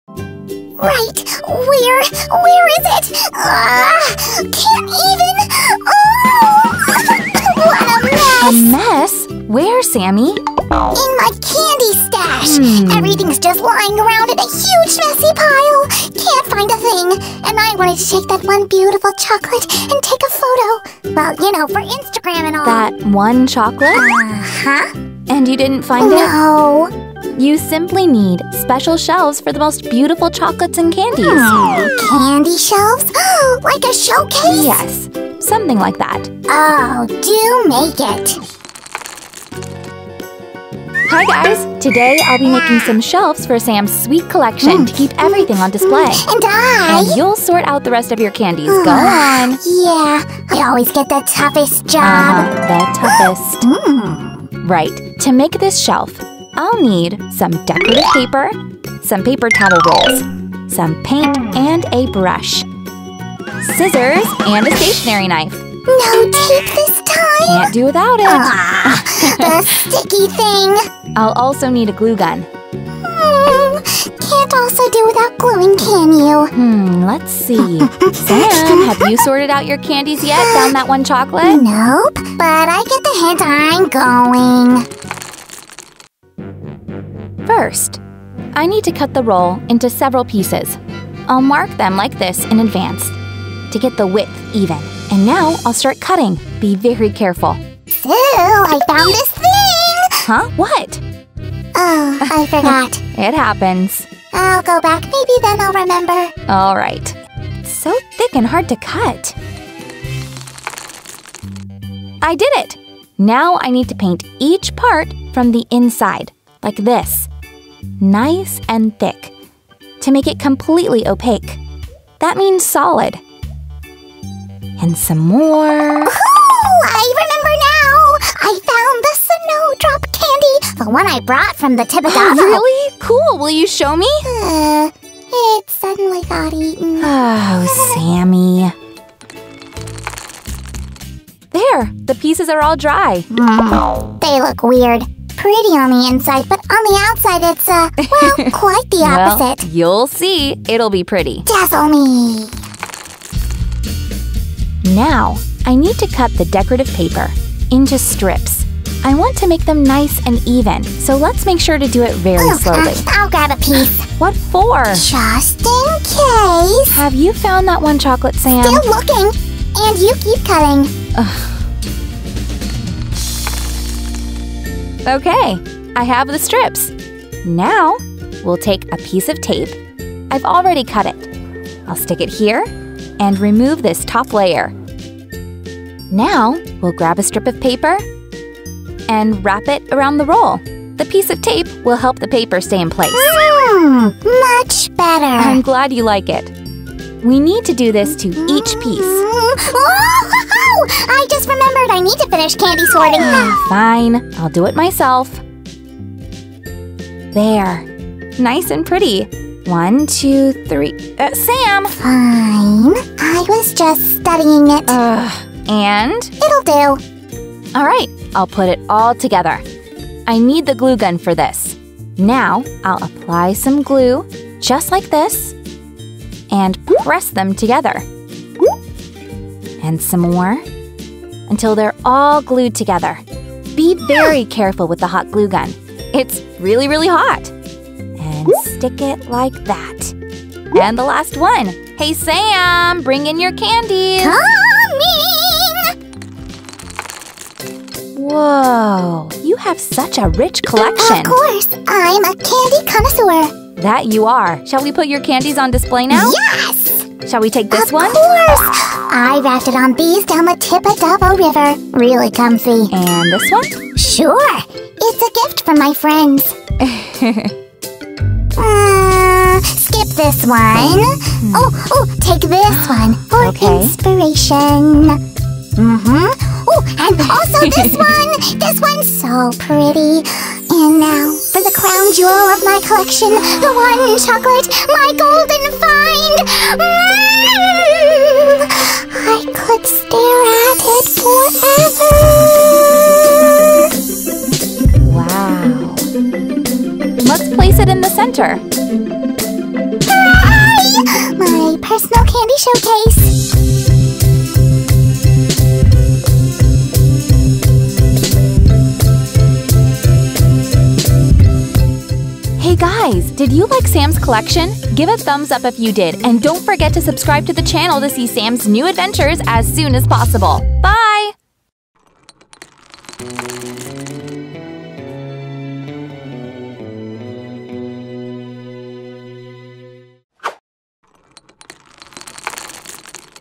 CANDY SHOWCASE for Slick Slime Sam
Sam has an impressive collection of chocolates and candies, but he doesn't know how to properly store them. Sue helps him out by making a very special showcase. The only thing Sam has to do is to sort out his collection. Will he make it?

Be careful using hot glue or ask an adult for help.